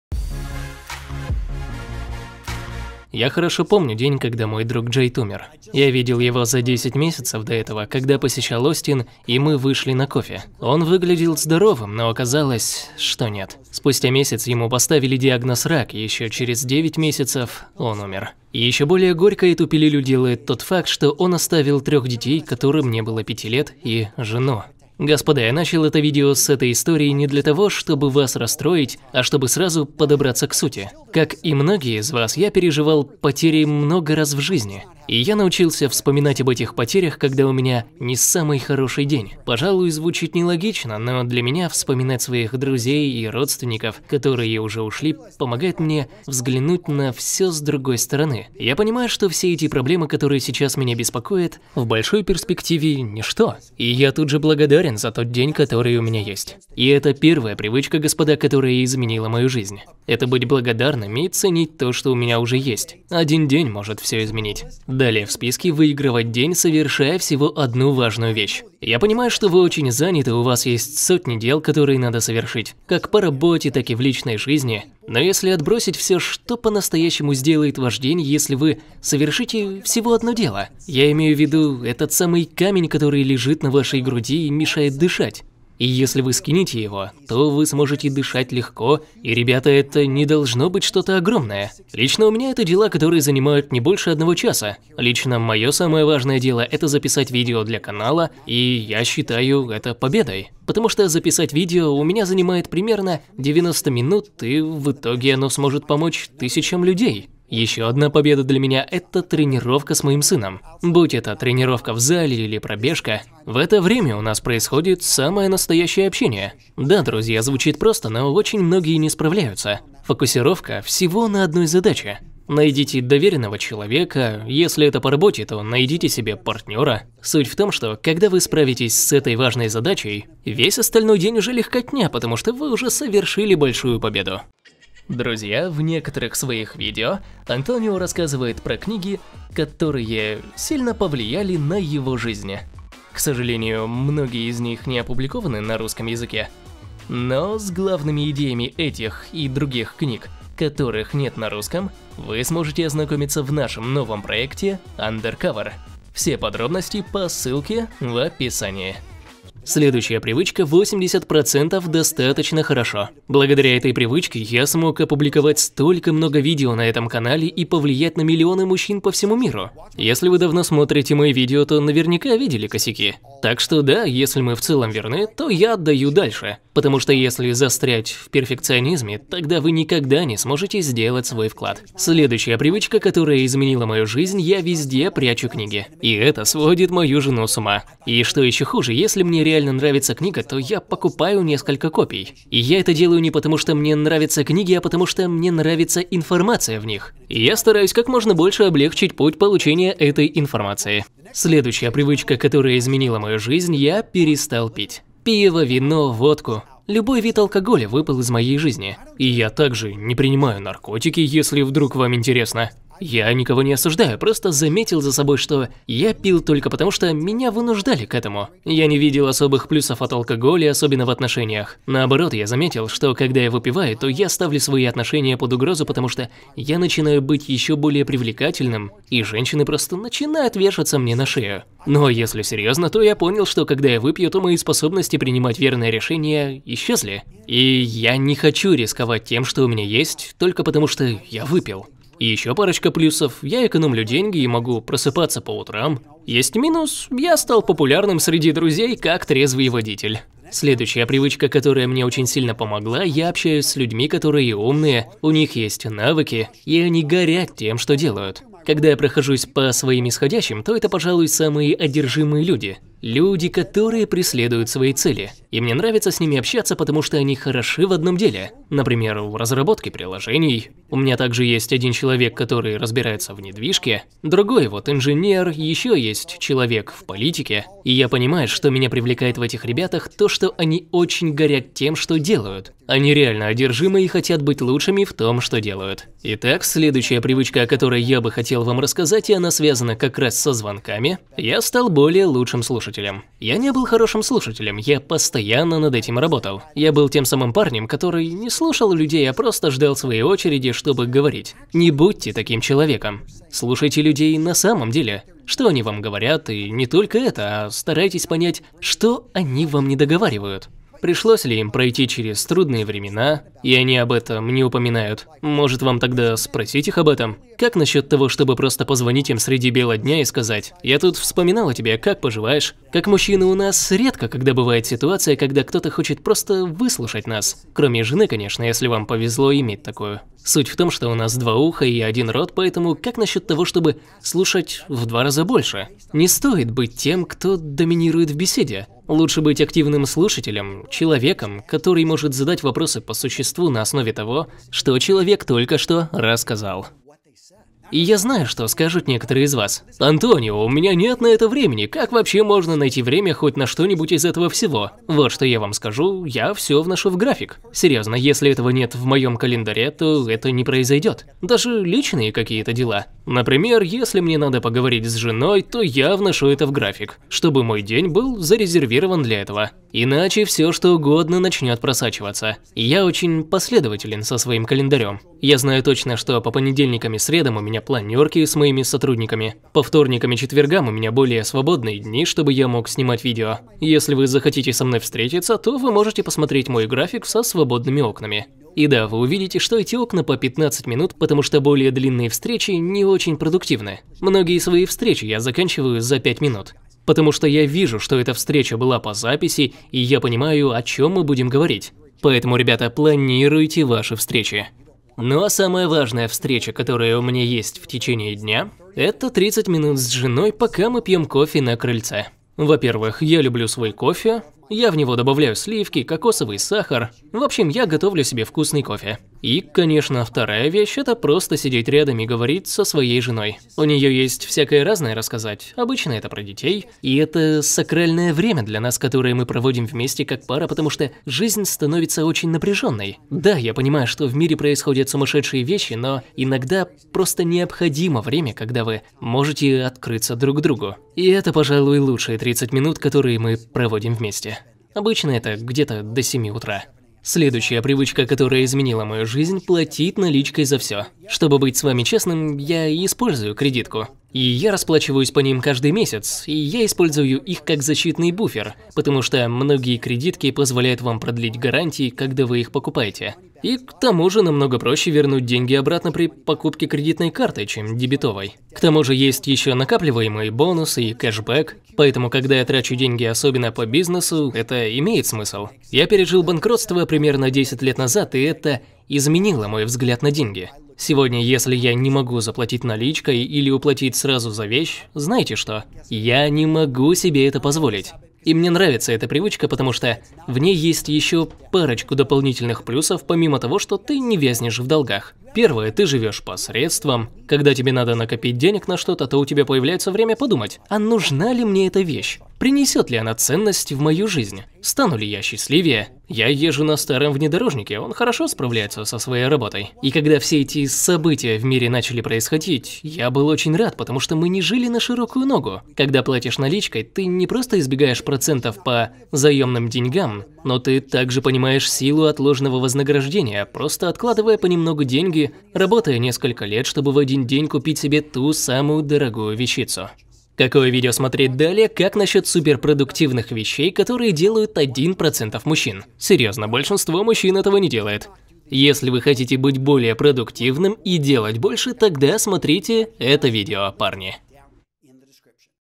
9 Привычек Миллионеров, Которые Изменили Мою Жизнь
Какие привычки нужно развивать для того, чтобы ответить себе, как стать успешным. Эти привычки миллионеров помогли Антонио Сентено изменить свою жизнь. Ведь привычки успешных людей – это полезные привычки.

9 Привычек Миллионеров, Которые Изменили Мою Жизнь

Ваше саморазвитие обусловлено приобретением различных привычек. Конечно, у вас должна быть для всего мотивация. Если хотите быть успешным, то необходимо развивать в себе привычки богатых людей.

Эти привычки богатых и успешных людей в любом случае дадут вам преимущество перед остальными в гонке за желаемым.

0:00 Начало
0:56 Быть благодарным
1:04 Всего одна важная вещь в день
2:39 80% достаточно хорошо
3:01 Везде прятать книги
3:21 Перестать пить
4:10 Общаться с умными людьми
4:46 Стать хорошим слушателем
5:55 График
7:27 Плати наличкой